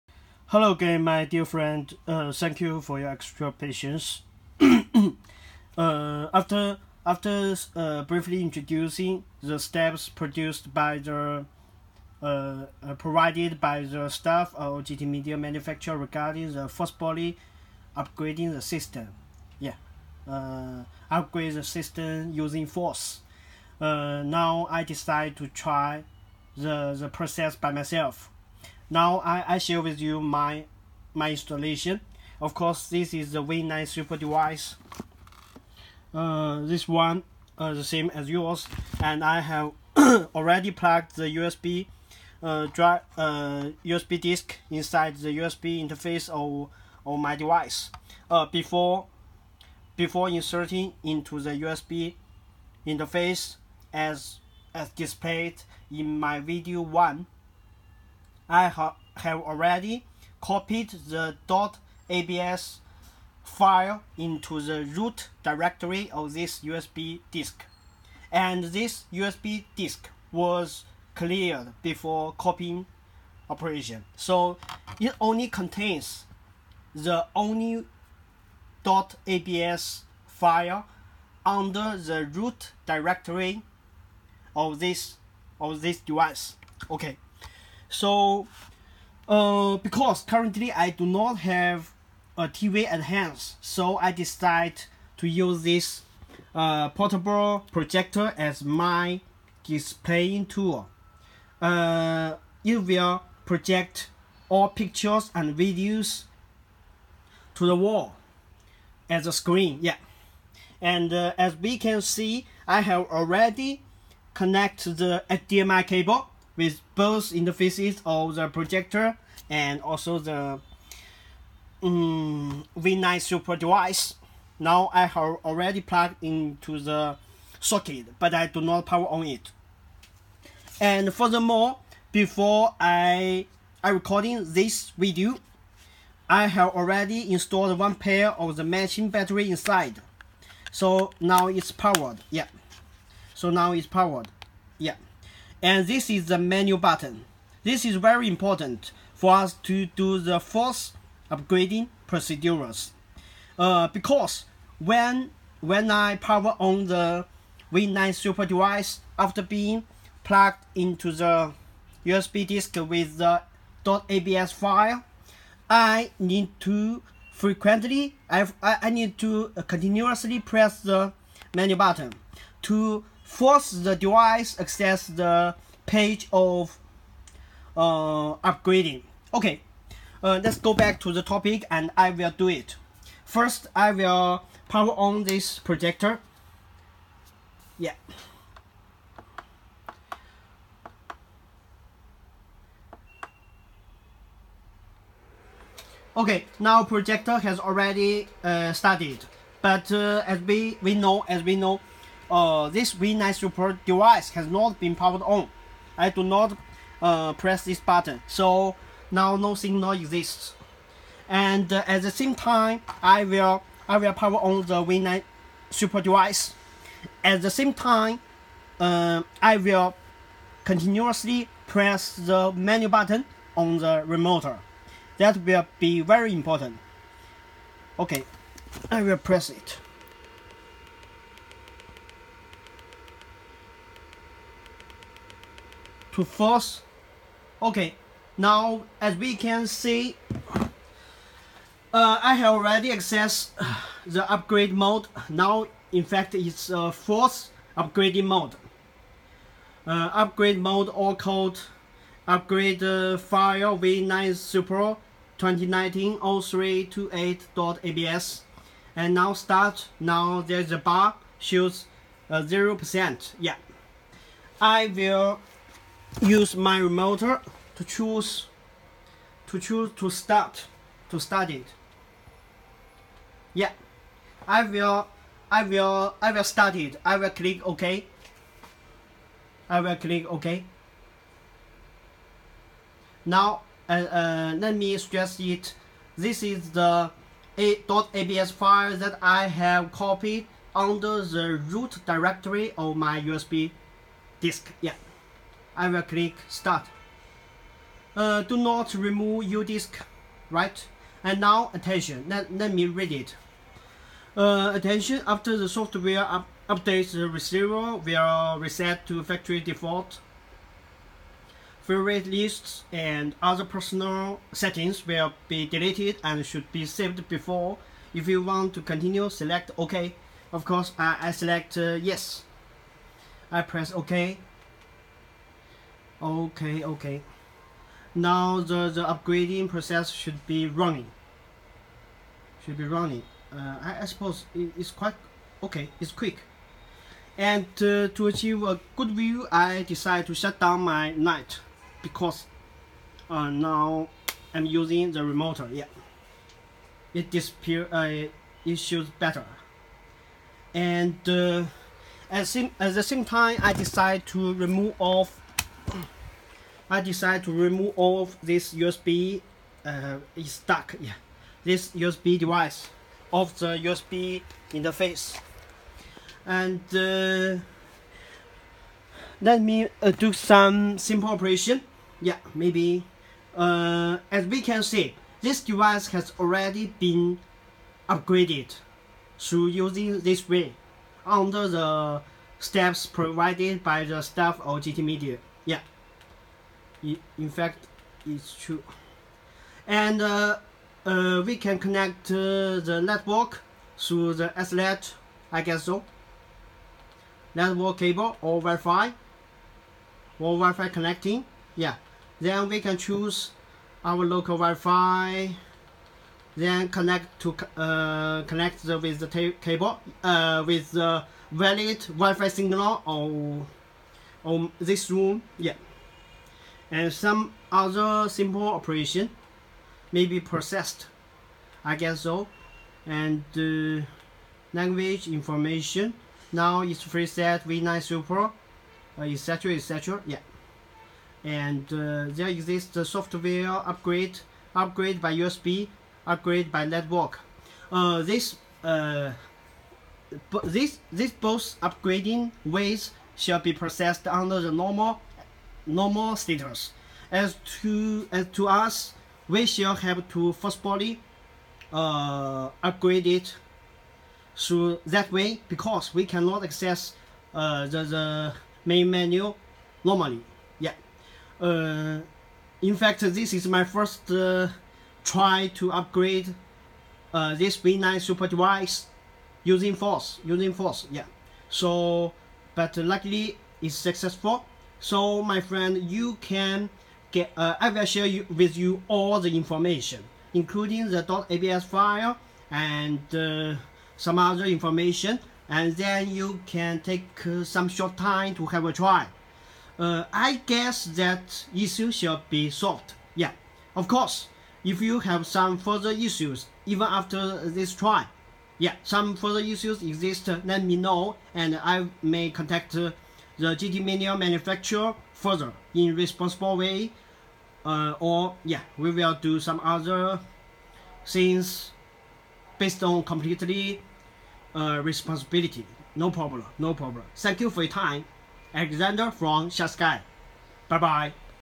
Video 2. My personal try on FORCE UPGRADE operation to recover system of GTEMDIA V9 SUPER receiver
In this video, I specially made a try of FORCE UPGRADE operation on GTMEDIA V9 SUPER device by myself, following steps provided by GTMEDIA staff. It took minutes to complete the process. Involved info was pre-introduced in Video 1.